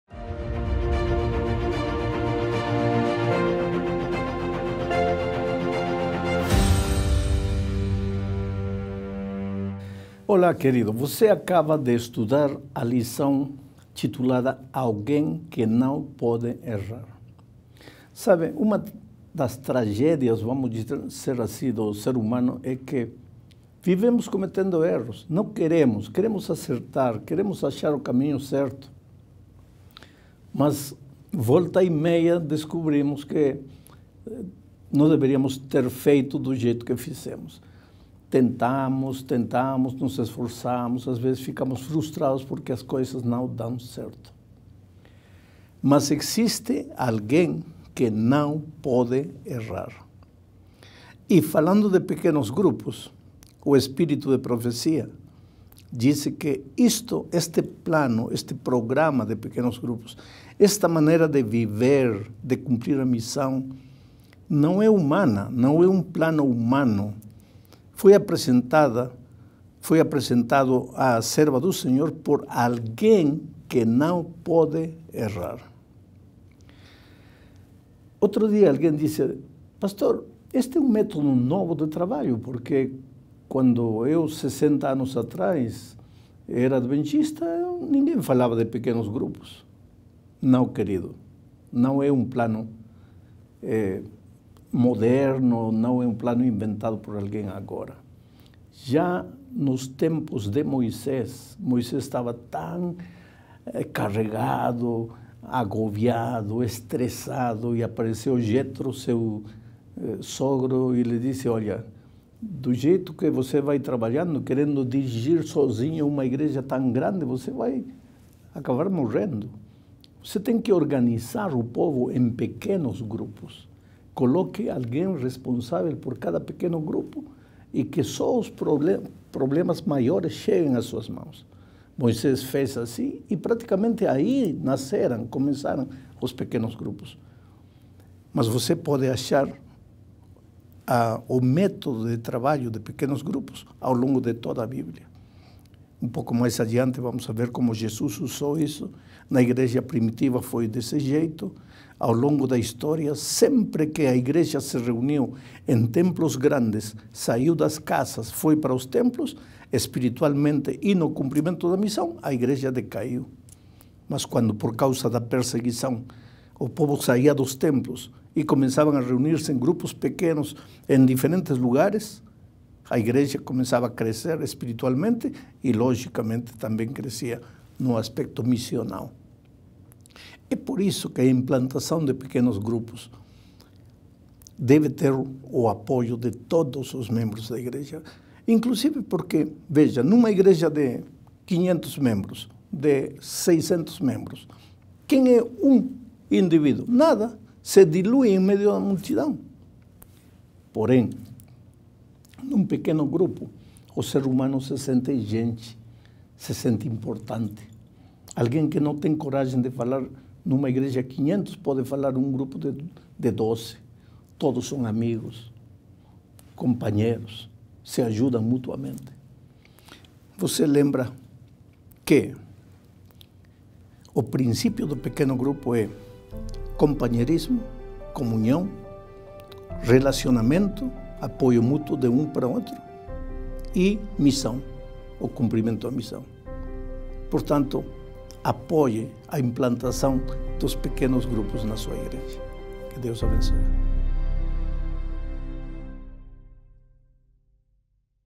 Alguém não pode errar | Implantação de Pequenos Grupos
Primeira lição do Curso de Implantação de Pequenos Grupos com o Pr. Alejandro Bullón.

FELIZ7PLAY